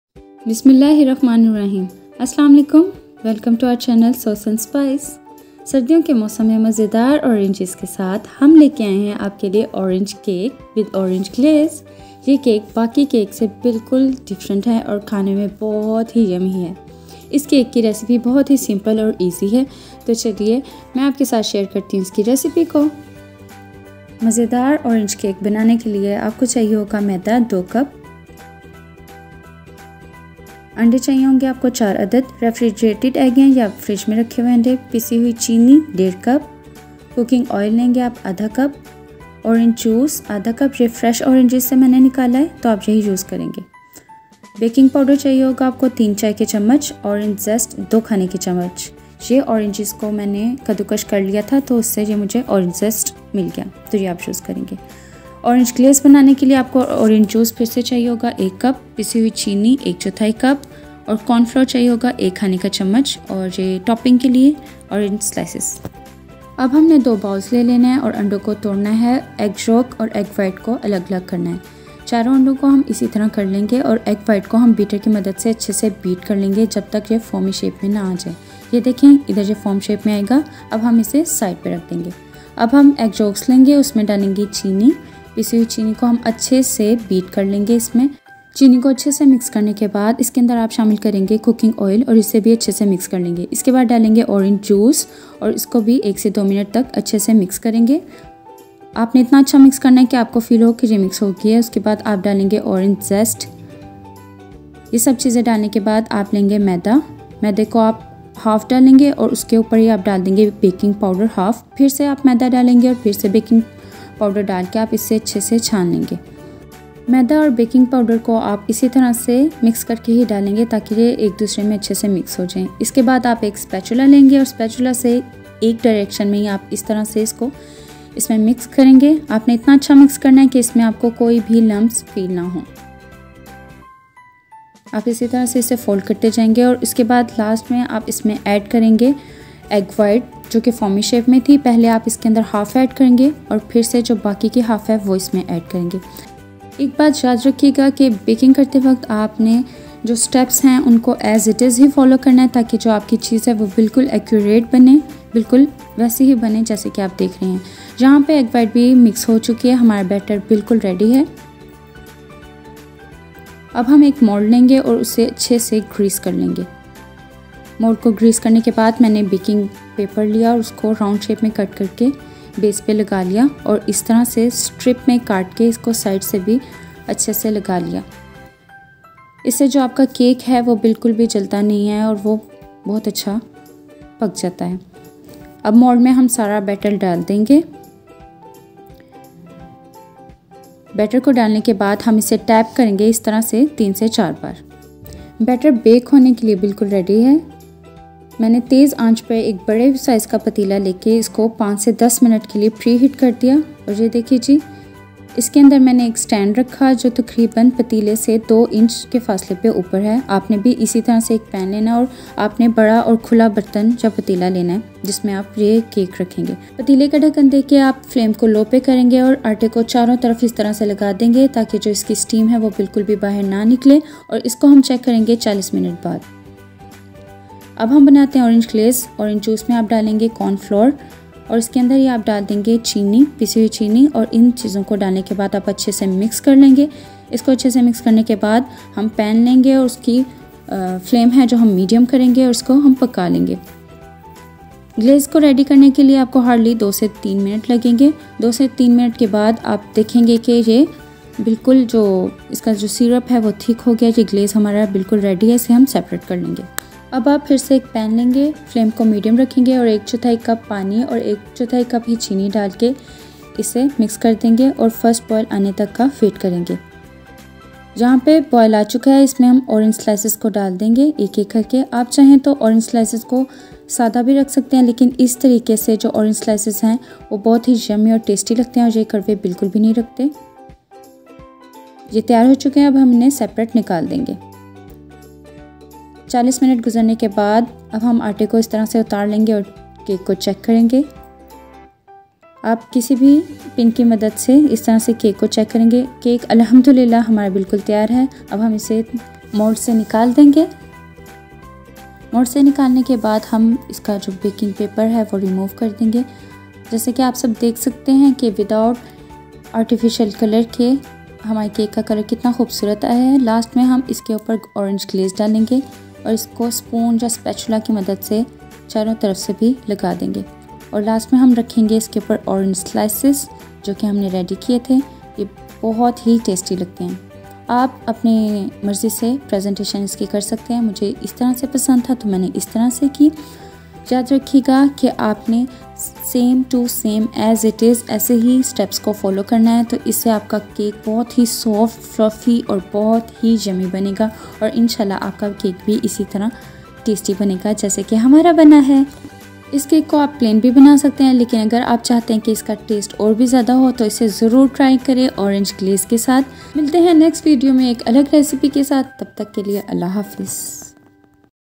Orange Cake Recipe | orange glaze| Without oven and artificial colour| Easy to Make
Hi guys! today's recipe is Orange Cake which is delicious and mouth watering. so watch our recipe and must try it cuz its very simple and easy. make sure to give us your feed back in comments. thanks